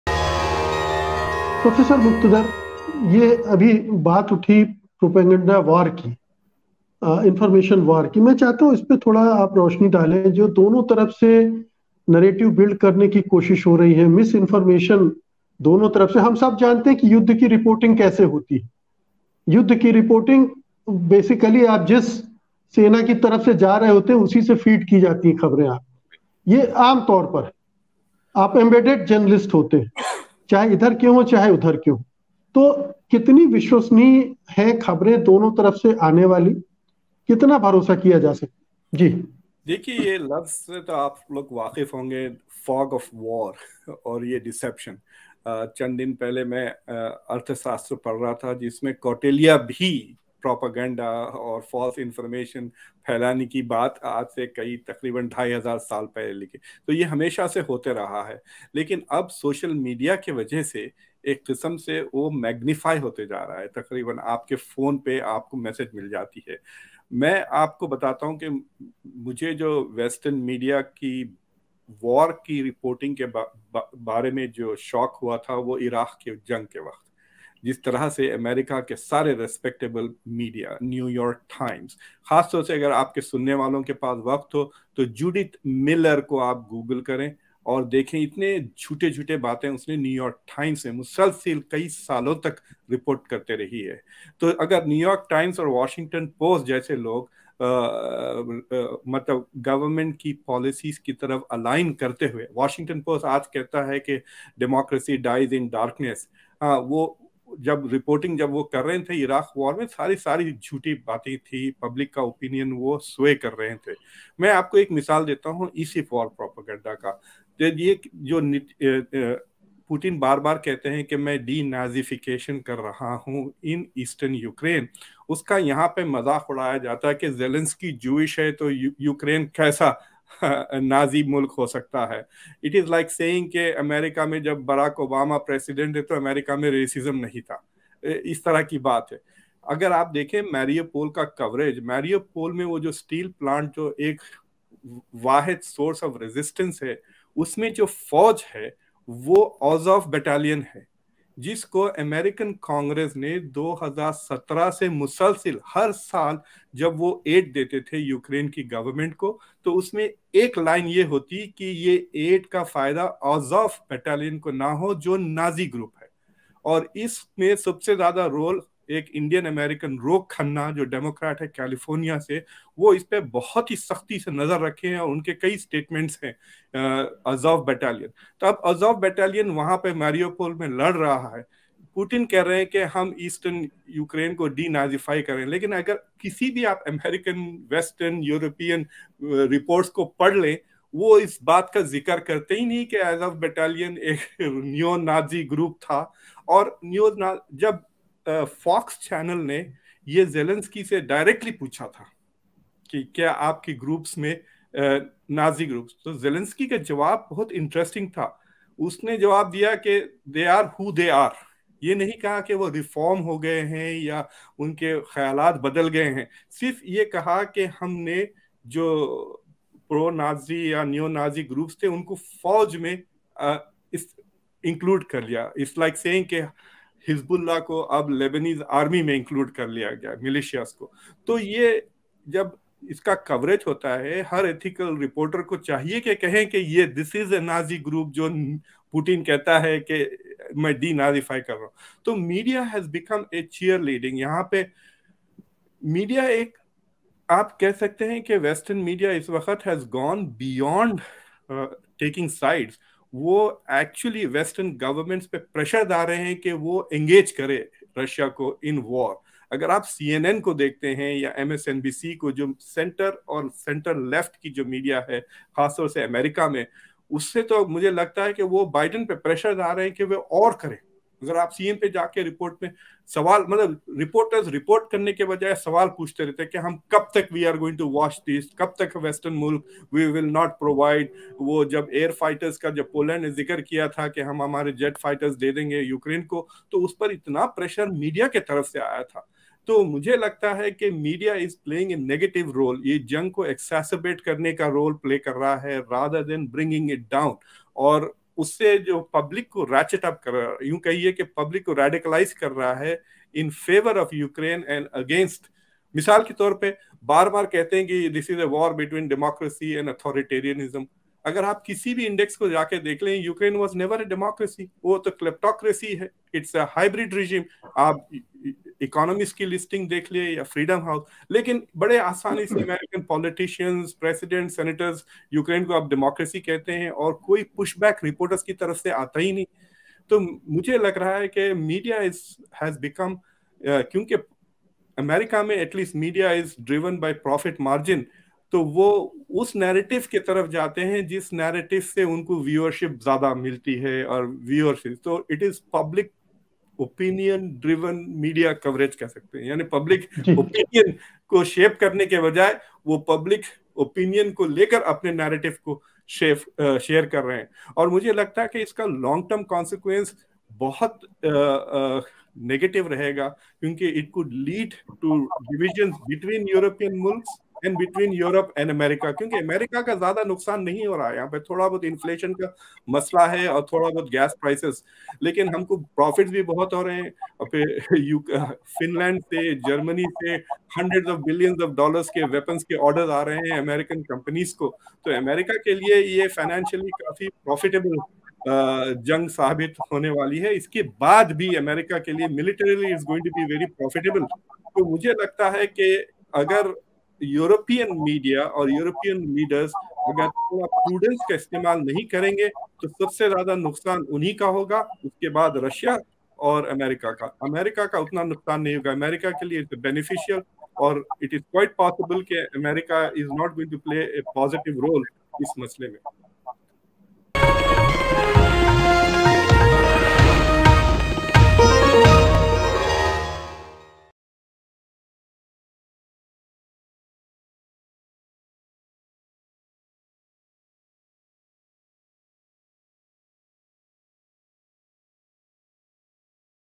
Is the media playing a role of further escalating the war?। VICTORY DAY । UKRAINE CRISIS
रूस-यूक्रेन युद्ध में मीडिया क्या कर रहा? क्या जंग को मीडिया और भड़का रहा है? क्या है जंग में पश्चिमी मीडिया की भूमिका?

What is the media doing in the Russo-Ukraine war? Is the media inciting the war further? What is the role of western media in the war?